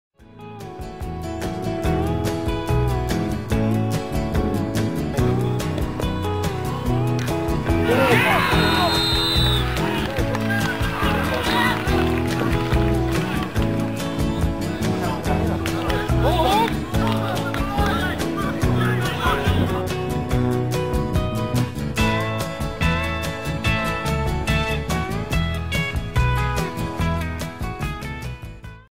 Rillandia - Colijnsplaatse Boys 1-1 (Nigel Holmes)
Zaterdag 10 November, Nigel Holmes is vanuit een vrije trap trefzeker tijdens de wedstrijd Rillandia - Colijnsplaatse Boys. Het filmpje is van de 1-1, het duel zou uiteindelijk eindigen in een 5-1 zege voor Rillandia.